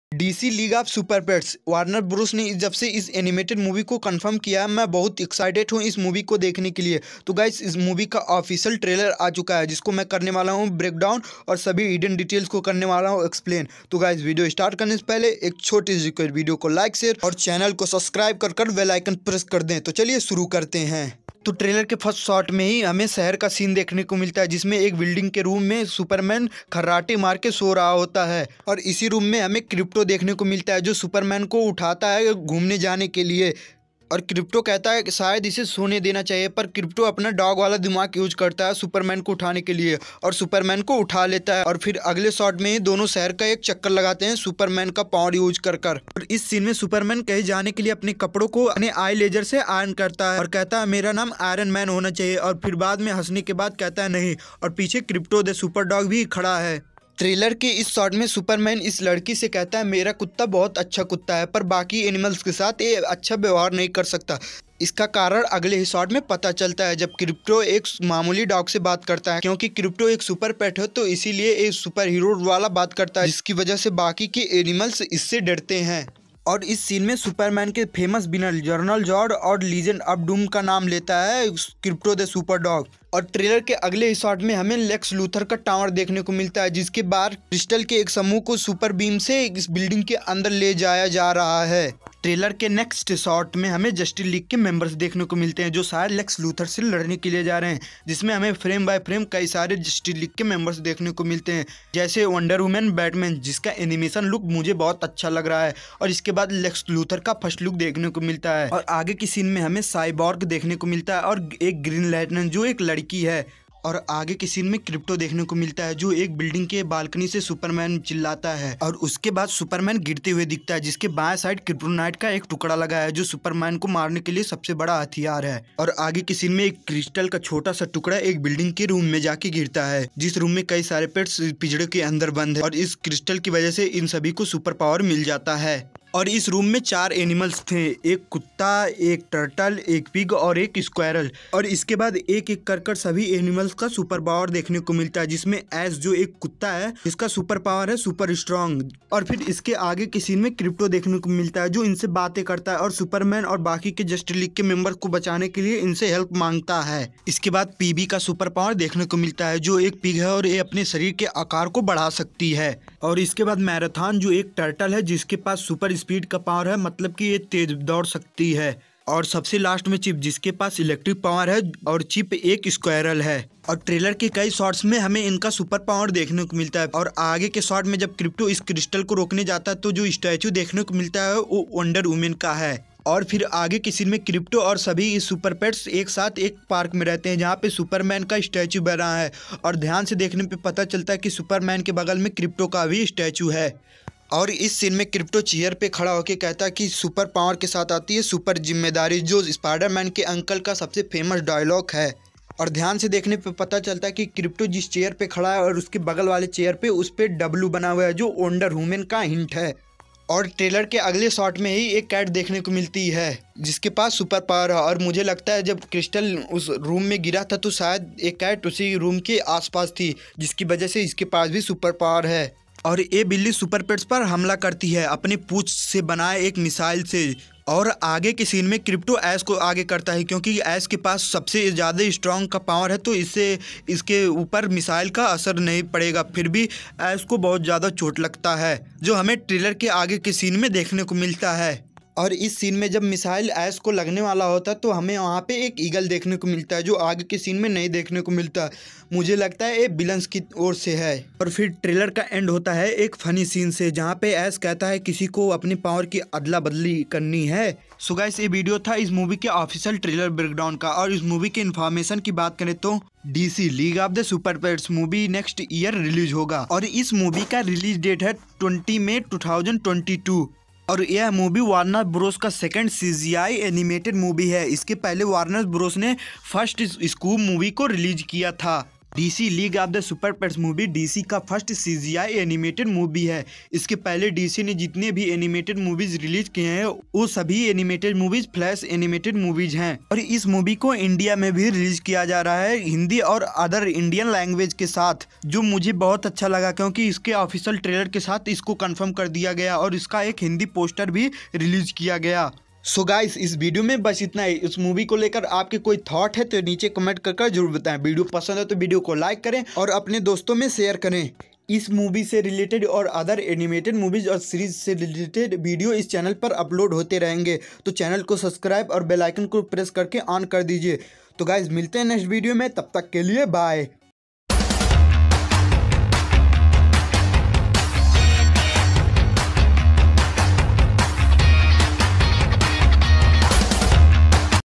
DC LEAGUE OF SUPERPETS - Official Trailer Breakdown in Hindi | Animated Movie
about- DC League of Superpets Brand New Animated movie of DC Movies A New Animated movie of Warner Bros Animation And Second CGI Animated movie of Warner Bros This movie is comic book carecters besued a super heroes animated movie
it's release next in hindi language.

create- DC Stuy.